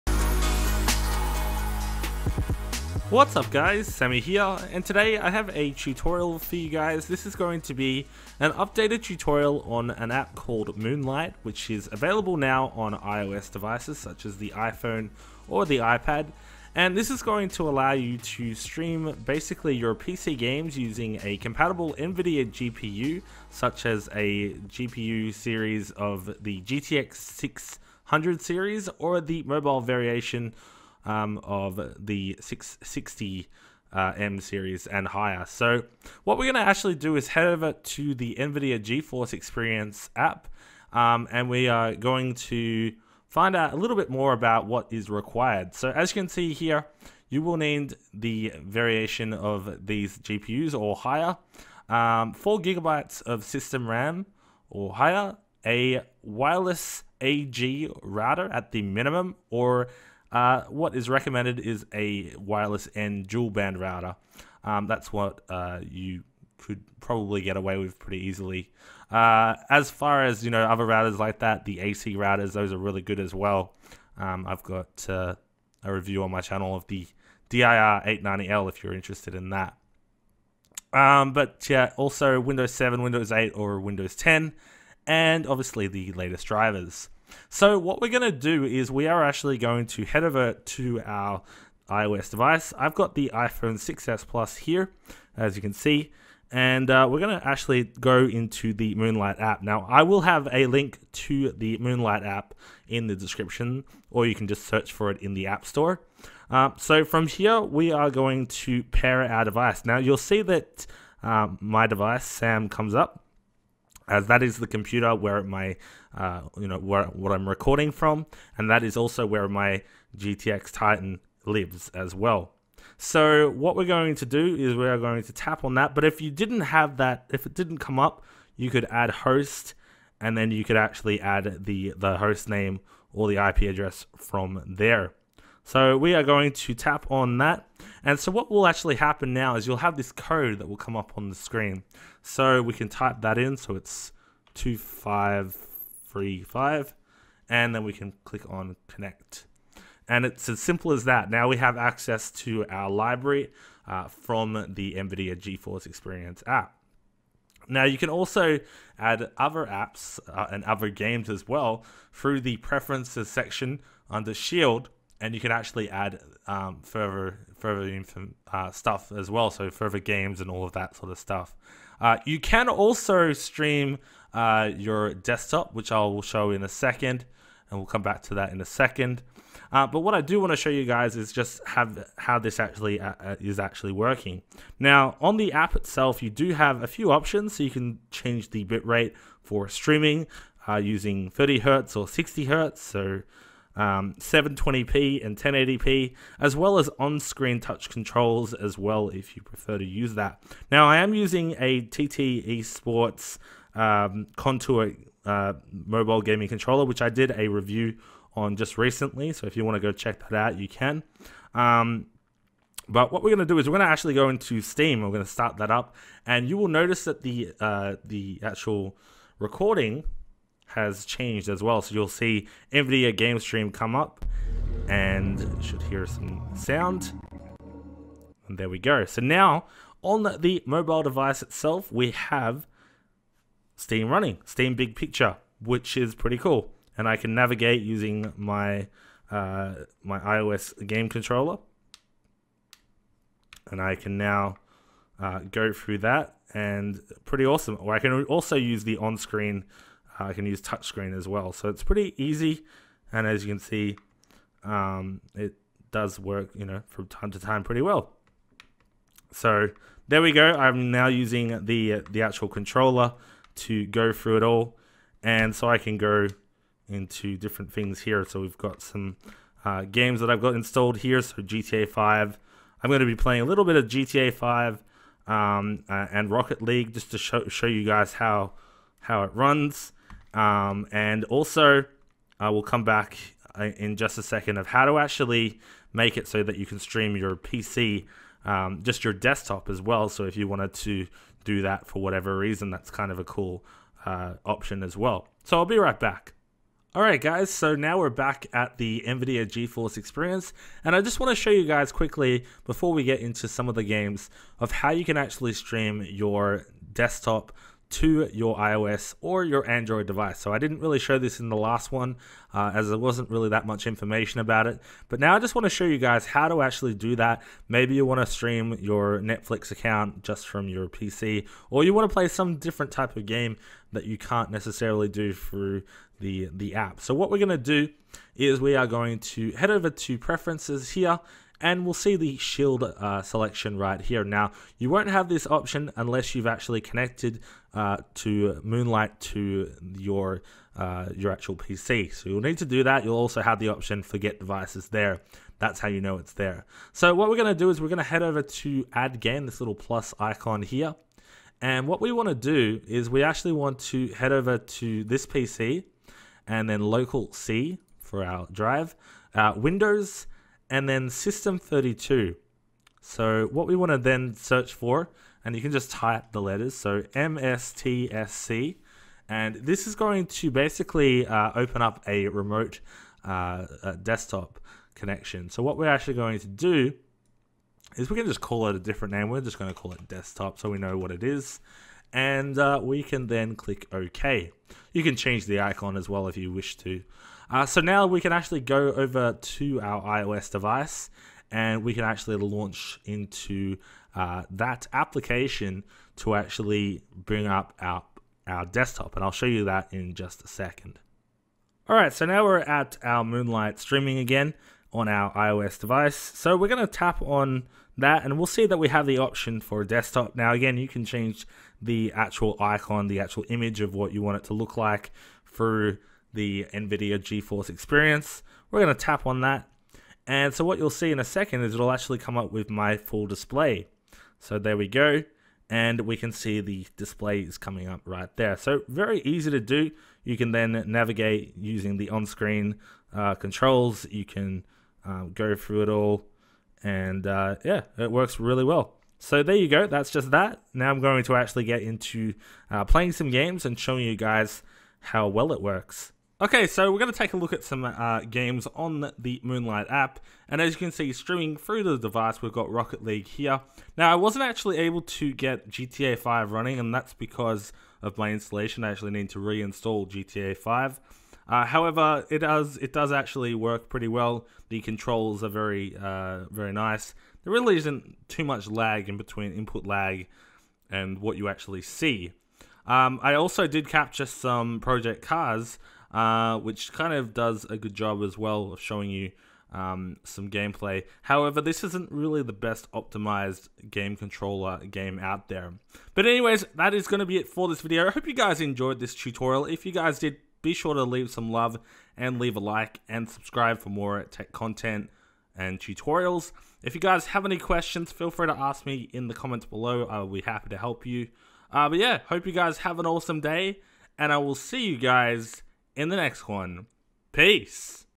Moonlight Game Streaming to iOS Devices - Guide
FAQ:

Q: "Unable to connect to the specified computer. Make sure the required ports are allowed through the firewall" Comes up?
A: You may need to forward some ports for your wireless router. Instructions can be found usually on Google (I cannot help you any further for this step as everyone has a different modem/wireless router). The required ports are as follows:

TCP: 35043, 47984, 47989, 47995, 47996, 48010
UDP: 47998, 47999, 48000

Q: Which iOS game controllers can I use with this?
A: There are many available out there. You can find a full list located

Q: Can I use a PS4/PS3 or Xbox One/Xbox 360 controller.
A: Not too sure about Xbox controllers but you'd probably require a jailbroken iOS device and a tweak called "Controllers For All" (can't confirm).